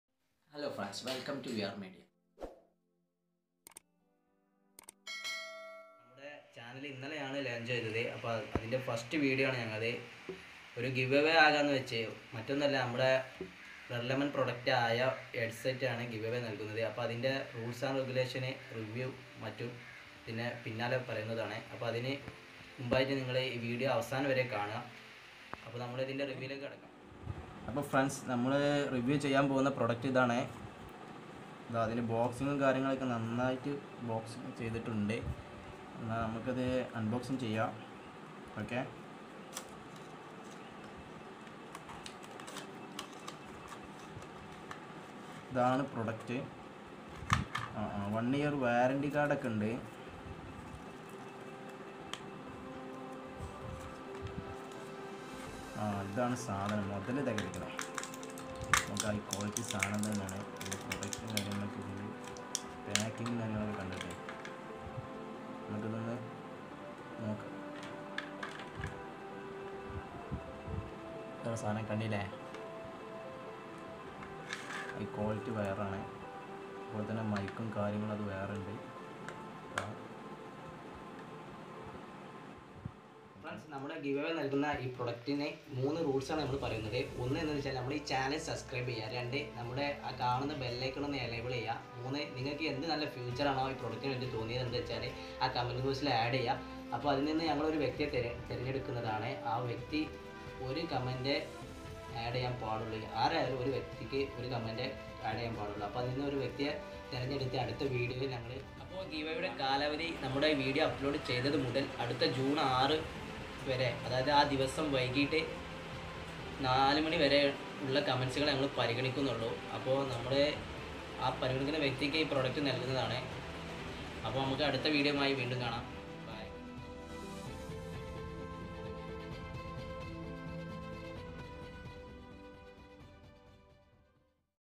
Giveaway | Red Lemon Sur Plus W120
(ഒരു കമന്റ്). തിരഞ്ഞെടുക്കുന്ന ഒരാൾക്ക് Red Lemon Head Headphone സമ്മാനമായി ലഭിക്കും.